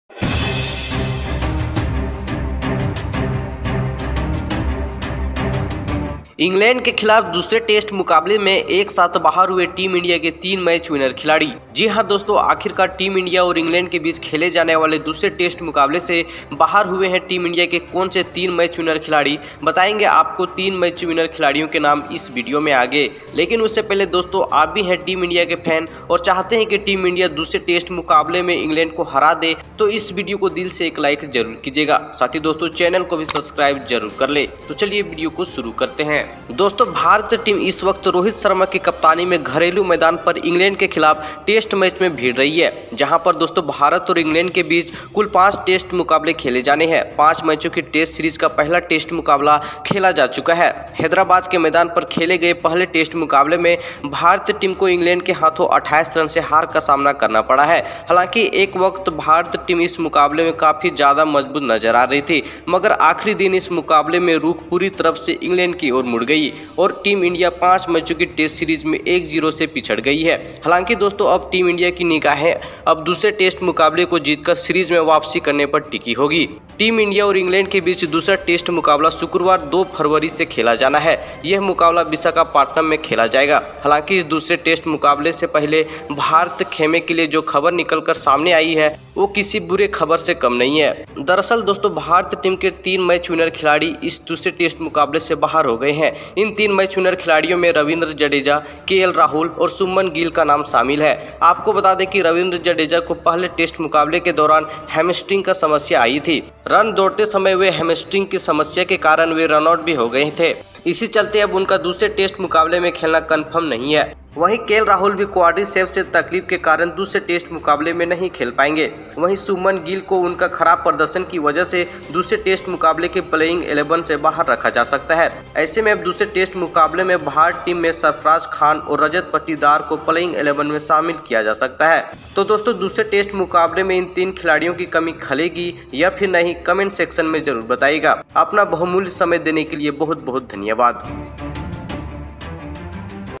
तीन मैच विनर खिलाडी बाहर | सरफराज की हुई एंट्री | Ind vs eng 2nd Test Match | Sport Talk
OM ARYA
From - Hazaribagh Jharkhand ( India )